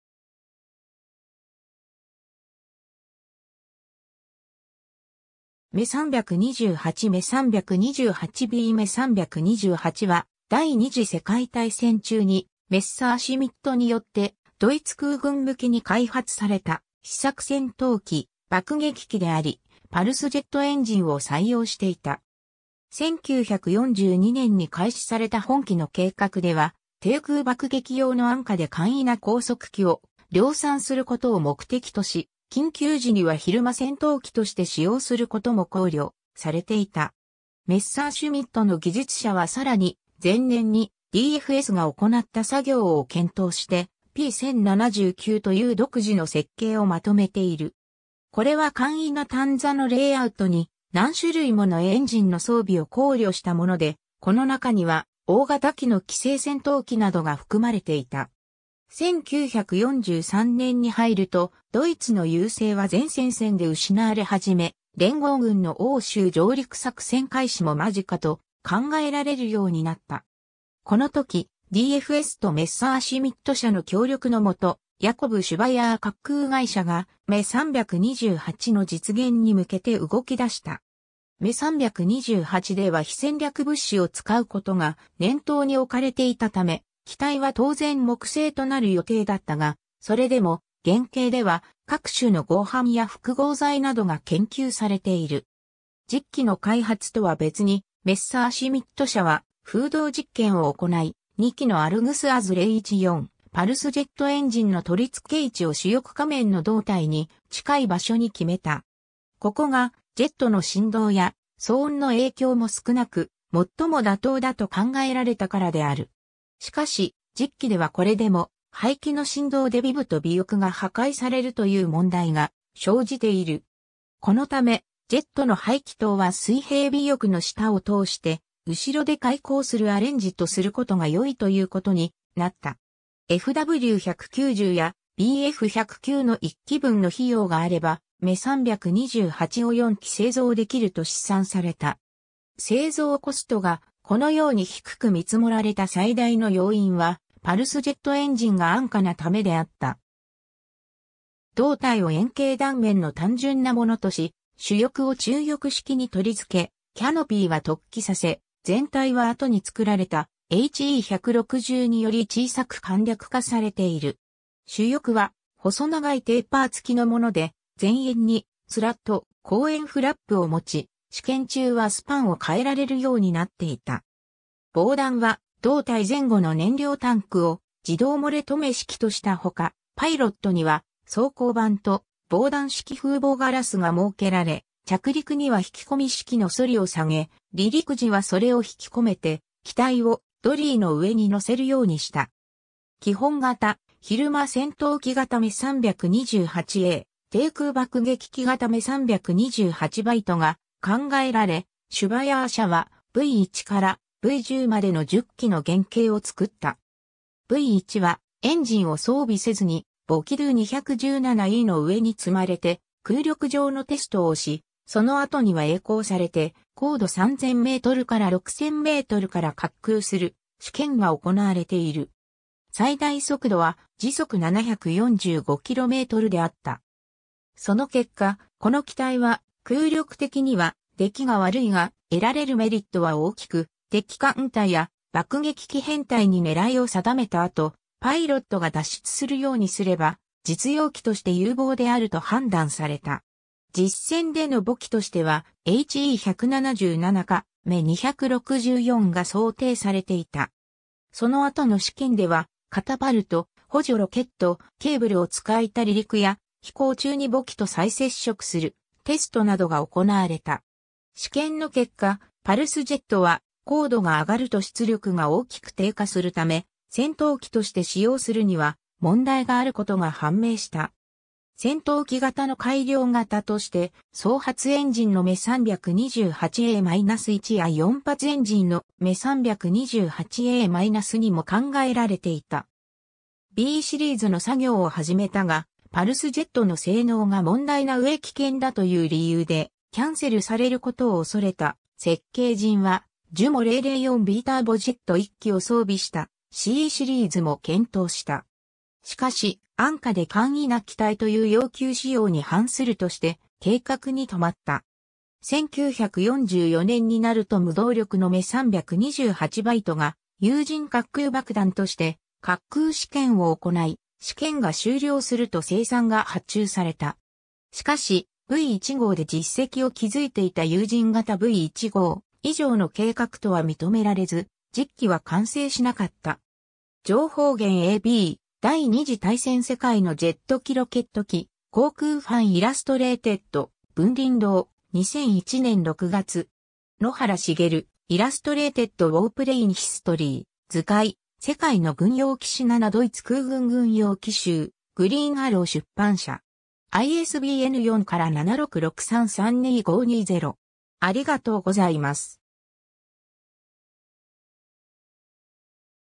Me 328 (航空機)
Me 328 Me 328 B Me 328（Messerschmitt Me 328）は、第二次世界大戦中にメッサーシュミットによってドイツ空軍向けに開発された試作戦闘機・爆撃機であり、パルスジェットエンジンを採用していた。
 1942年に開始された本機の計画では、低空爆撃用の安価で簡易な高速機を量産することを目的とし、緊急時には昼間戦闘機として使用することも考慮されていた。
メッサーシュミットの技術者はさらに、前年にDFSが行った作業を検討して、P.1079という独自の設計をまとめている。
これは簡易な単座のレイアウトに、何種類ものエンジンの装備を考慮したもので、このなかには大型機の寄生戦闘機などが含まれていた。
 1943年に入るとドイツの優勢は全戦線で失われ始め、連合軍の欧州上陸作戦開始も間近と考えられるようになった。
このとき、DFSとメッサーシュミット社の協力のもと、ヤコブ・シュヴァイアー滑空会社がMe 328の実現に向けて動きだした。
Me 328では非戦略物資を使うことが念頭に置かれていたため、機体は当然木製となる予定だったが、それでも原型では各種の合板や複合材などが研究されている。
 実機の開発とは別に、メッサーシュミット社は風洞実験を行い、2基のアルグス As 014パルスジェットエンジンの取り付け位置を主翼下面の胴体に近い場所に決めた。
ここがジェットの振動や騒音の影響も少なく、最も妥当だと考えられたからである。
しかし、実機ではこれでも排気の振動で尾部と尾翼が破壊されるという問題が生じている。
このため、ジェットの排気筒は水平尾翼の下を通して後ろで開口するアレンジとすることが良いということになった。
 Fw 190やBf 109の1機分の費用があれば、Me 328を4機製造できると試算された。
製造コストがこのように低く見積もられた最大の要因は、パルスジェットエンジンが安価なためであった。
 胴体を円形断面の単純なものとし、主翼を中翼式に取り付け、キャノピーは突起させ、全体は後に作られたHe 162より小さく簡略化されている。
 主翼は細長いテーパー付きのもので、前縁にスラット、後縁フラップを持ち、試験中はスパンを変えられるようになっていた。
防弾は胴体前後の燃料タンクを自動洩れ止め式としたほか、パイロットには装甲板と防弾式風防ガラスが設けられ、着陸には引き込み式のソリを下げ、離陸時はそれを引き込めて機体をドリーの上に載せるようにした。
 基本型、昼間戦闘機型Me 328A、低空爆撃機型Me 328Bが考えられ、シュヴァイアー社はV1からV10までの10機の原型を作った。
V1はエンジンを装備せずに母機Do 217E(JT+FL)の上に積まれて空力上のテストをし、その後には曳航されて高度3,000m～6,000mから滑空する試験が行われている。
最大速度は745km/hであった。
その結果、この機体は空力的には出来が悪いが、得られるメリットは大きく、敵艦隊や爆撃機編隊に狙いを定めたあと、パイロットが脱出するようにすれば、実用機として有望であると判断された。
実戦での母機としてはHe 177かMe 264が想定されていた。
 その後の試験では、カタパルト、補助ロケット、ケーブルを使いた離陸や、飛行中に母機と再接触するテストなどが行われた。
試験の結果、パルスジェットは高度が上がると出力が大きく低下するため、戦闘機として使用するには問題があることが判明した。
 戦闘機型の改良型として双発エンジンのMe 328A-1や4発エンジンのMe 328A-2も考えられていた。
 Bシリーズの作業を始めたが、パルスジェットの性能が問題なうえ危険だという理由でキャンセルされることを恐れた設計陣は、Jumo004Bターボジェット1基を装備したCシリーズも検討した。
しかし“安価で簡易”な機体という要求仕様に反するとして計画に留まった。
 1944年になると無動力のMe 328Bが有人滑空爆弾として滑空試験を行い、試験が終了すると生産が発注された。
しかしV1号で実績を築いていた有人型V1号以上の計画とは認められず、実機は完成しなかった。
 情報源 ^ a b 『第二次大戦世界のジェット機ロケット機』『航空ファン』イラストレイテッド、文林堂、2001年6月。
 ^ 野原　茂『ILLUSTRATED WARPLANE HISTORY 【図解】世界の軍用機史７　ド...